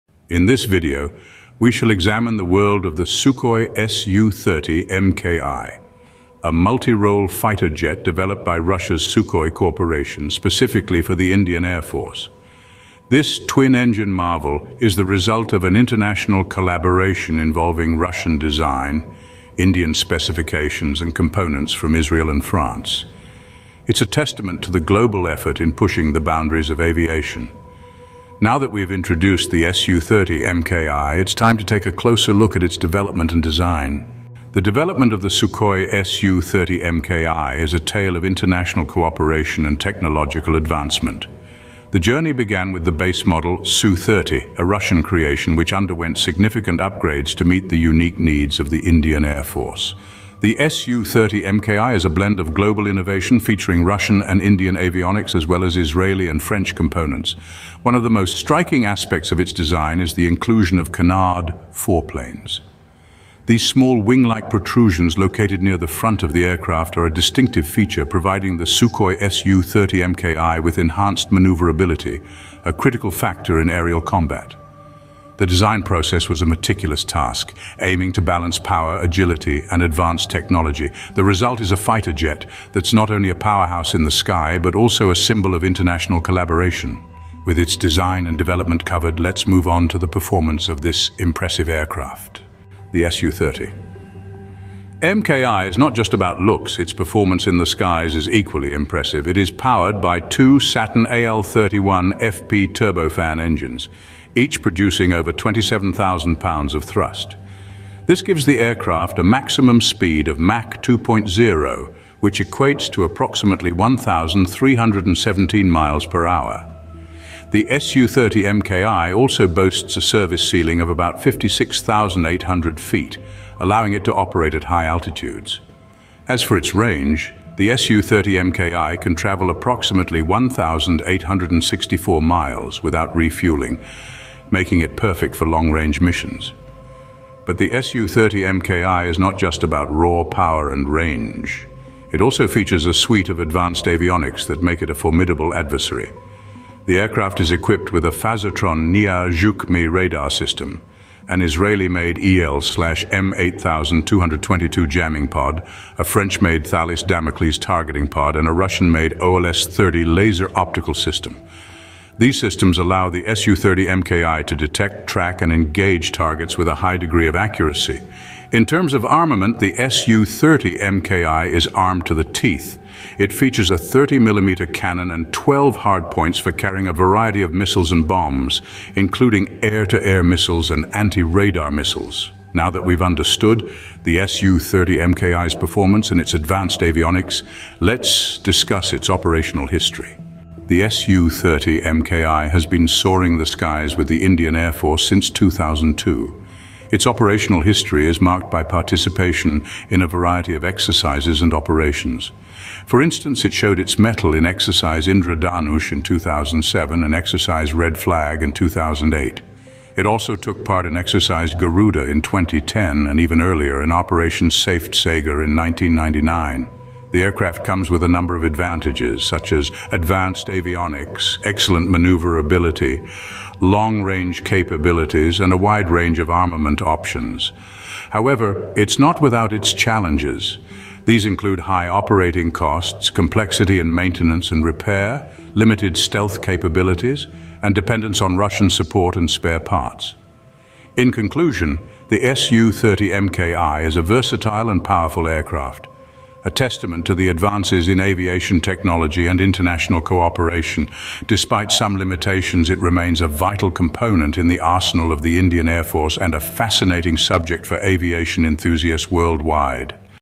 Why India Chose Russia's Su-30MKI Over the Rafale
Here are the operational capabilities of the Su-30MKI fighter jet:

 Air-to-Air Capabilities:
1. Beyond Visual Range (BVR) combat: The Su-30MKI is equipped with the Phazotron NIIR Zhuk-ME radar system, which enables BVR combat.

2. Air-to-air missile capability: The Su-30MKI can carry a range of air-to-air missiles, including the R-77 (AA-12) and R-73 (AA-11).

3. Dogfighting: The Su-30MKI's advanced aerodynamics and thrust-vectoring engines make it highly maneuverable in close combat.

 Air-to-Ground Capabilities:
1. Precision-guided munitions: The Su-30MKI can carry a range of precision-guided munitions, including laser-guided bombs and TV-guided missiles.

2. Ground attack: The Su-30MKI's advanced radar and targeting systems enable it to conduct effective ground attacks.

3. Close air support: The Su-30MKI's maneuverability and precision-guided munitions make it an effective close air support platform.

Electronic Warfare Capabilities:

1. Radar jamming: The Su-30MKI is equipped with an Israeli-made EL/M-8222 jamming pod, which enables it to conduct radar jamming.

2. Electronic countermeasures: The Su-30MKI has advanced electronic countermeasures (ECMs) to protect itself from enemy radar and missiles.

Other Capabilities:

1. Aerial refueling: The Su-30MKI is capable of aerial refueling, which extends its range and endurance.

2. Advanced avionics: The Su-30MKI has advanced avionics, including a glass cockpit and a range of sensors and systems.

3. Multi-role capability: The Su-30MKI is a multi-role fighter, capable of conducting a range of missions, including air-to-air, air-to-ground, and electronic warfare.